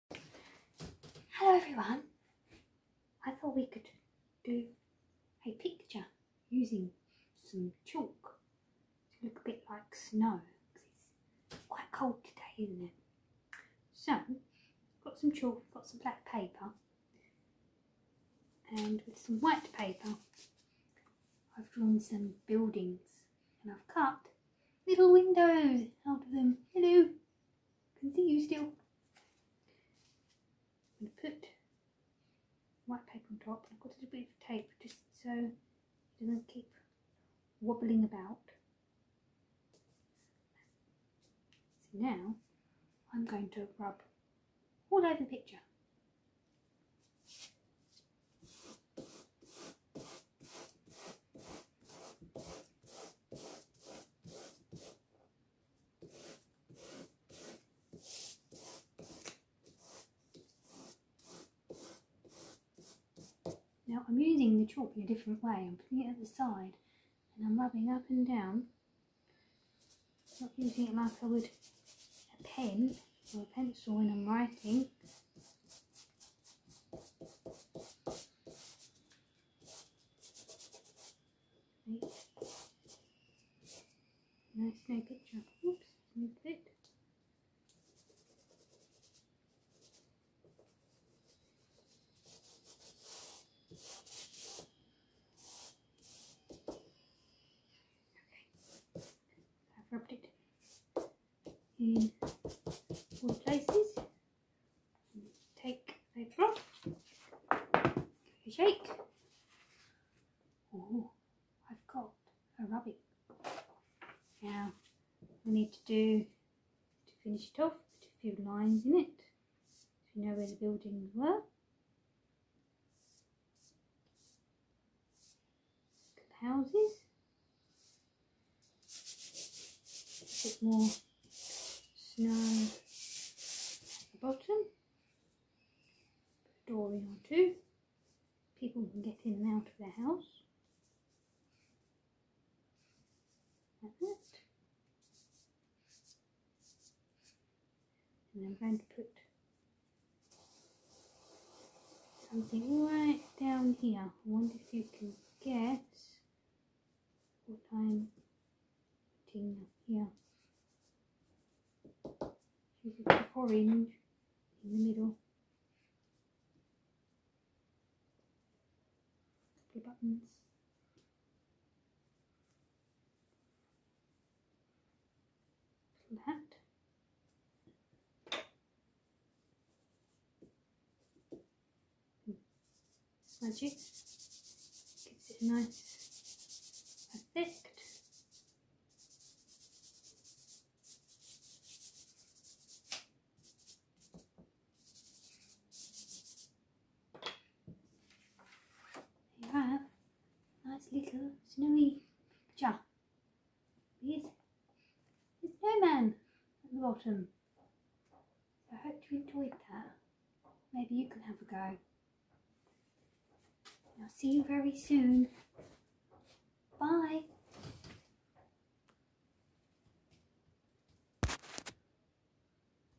Chalk pictures
Making winter scenes with Miss Smith.